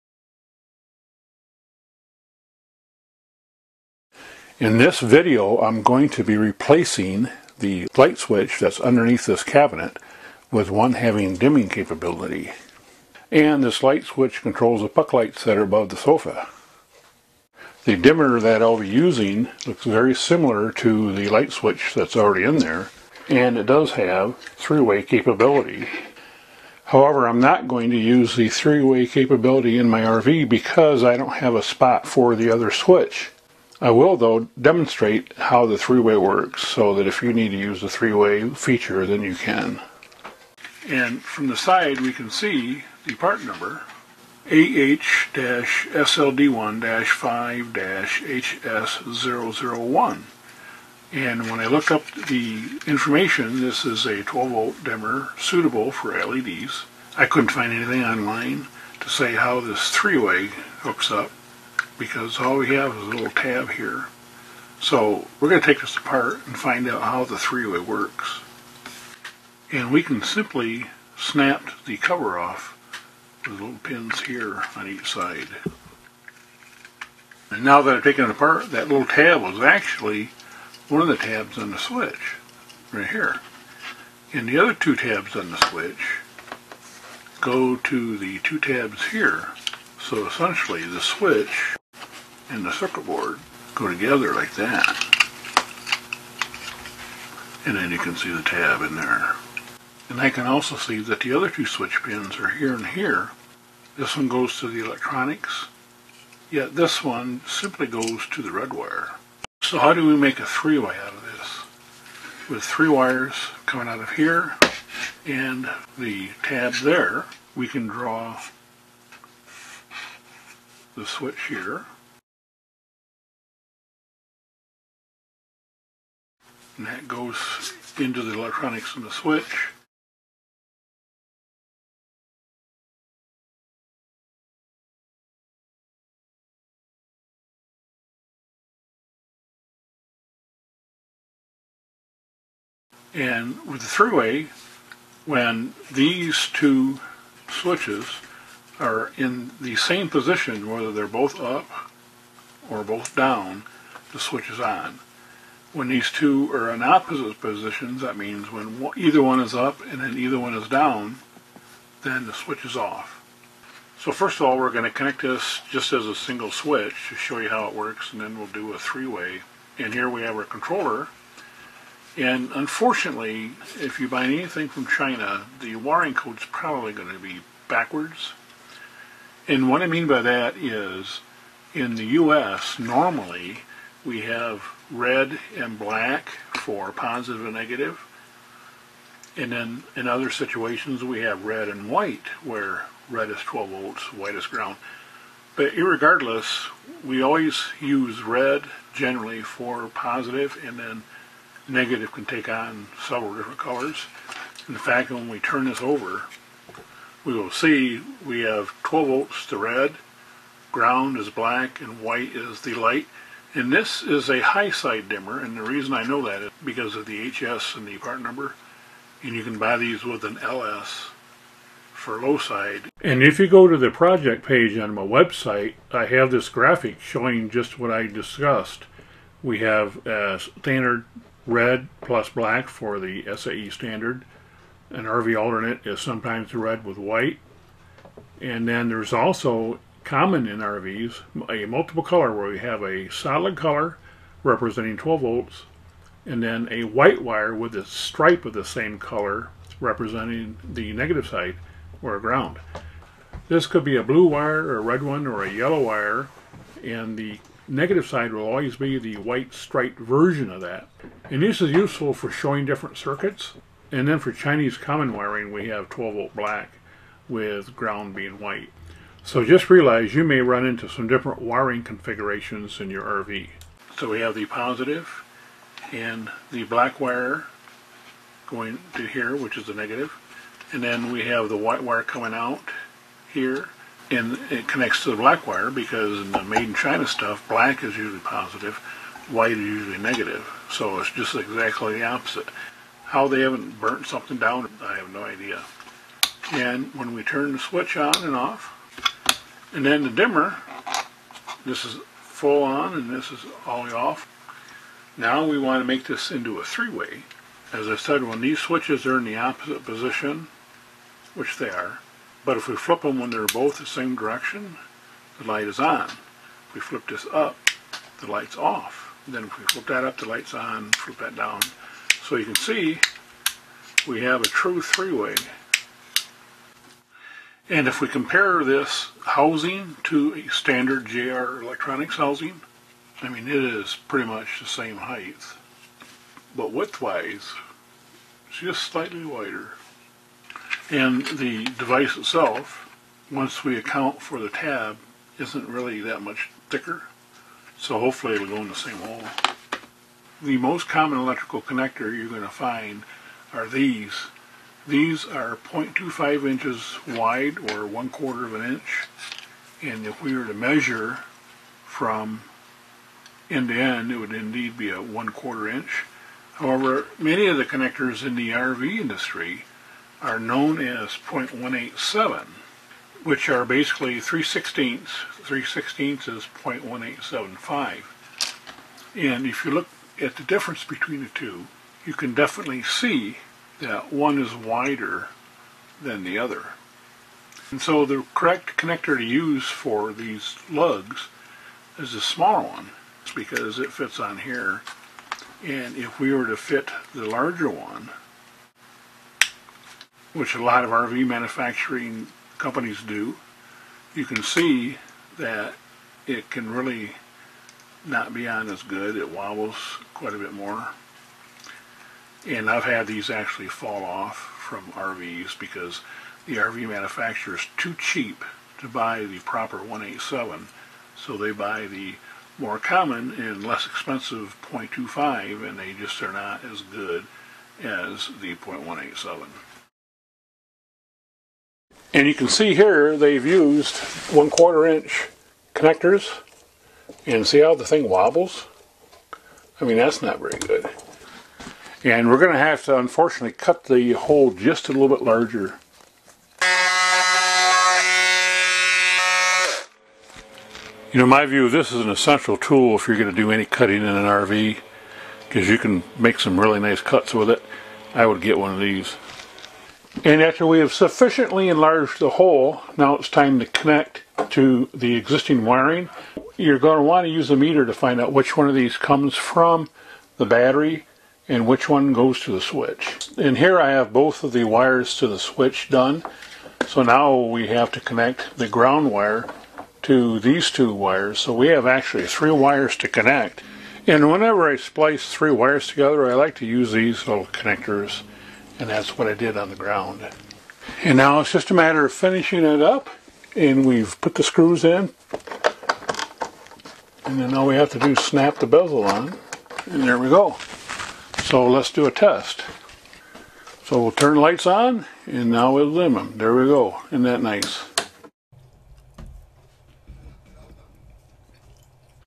Installing a LED dimmer in my RV
In this video, I install a 3-way capable LED dimmer in my RV.

advertisement
Don't pay $85 for the dimmer.
You can buy the dimmer here for much less
SPDT 3 way switch